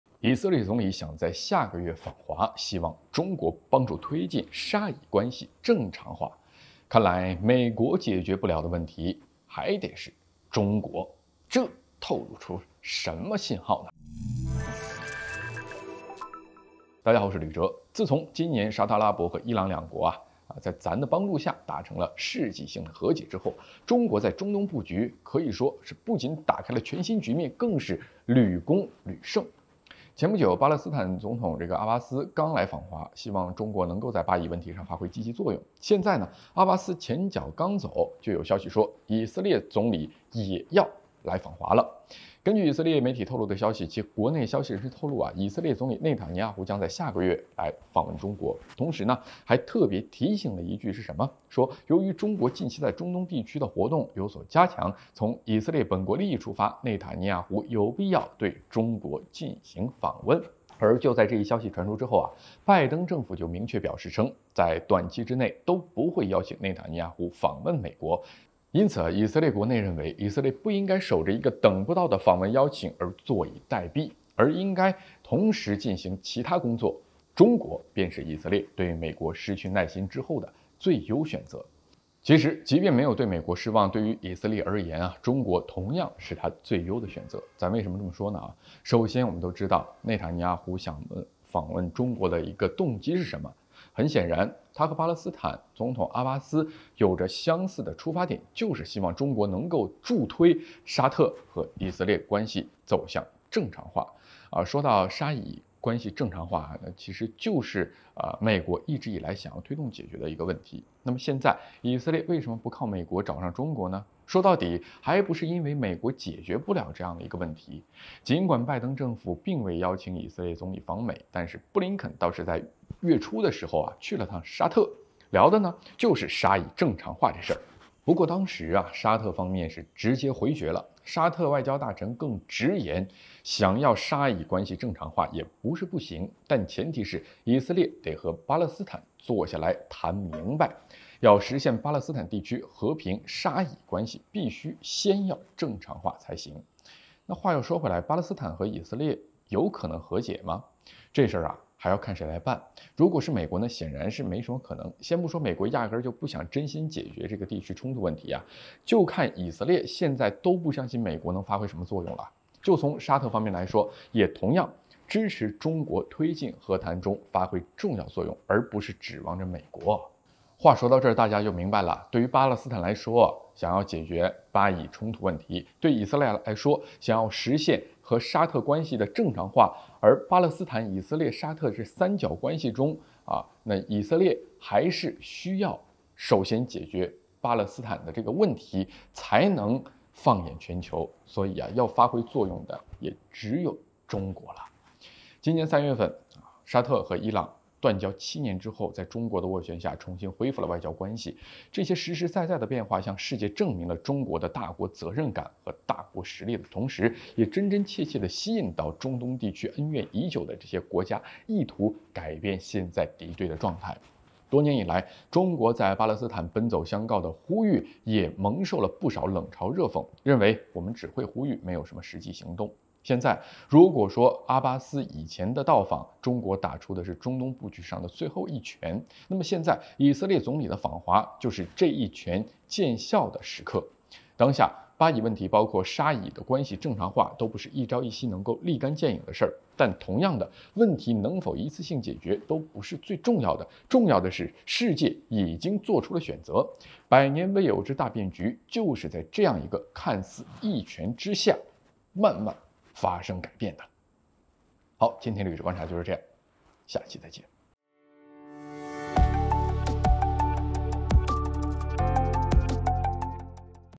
以色列總理下月訪華，中國借巴勒斯坦打出的一拳，起了效果【吕喆】
以色列總理想在下月訪華，希望中國幫助推進沙以關係正常化。美國解決不了的問題，找上了中國，透露了什麼信號？
以色列總理下月訪華，中國借巴勒斯坦打出的一拳，起了效果
刘晓非说 軍事 我們的影片會遵循公平、尊重的原則，確保不會包含任何仇恨言論的元素。